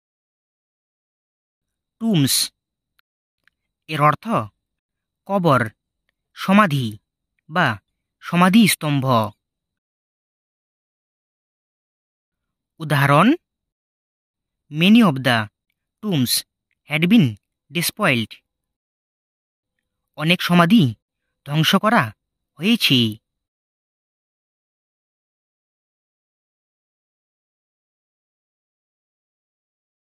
Tombs Meaning in Bengali With Example || Tombs শব্দটির বাংলা অর্থ কি হবে ?
এই ভিডিওতে tombs শব্দটির বাংলা অর্থ উদাহরণ সহকারে আলোচনা করা হয়েছে। আশা করছি ভিডিওটি আপনাদের উপকারে আসবে।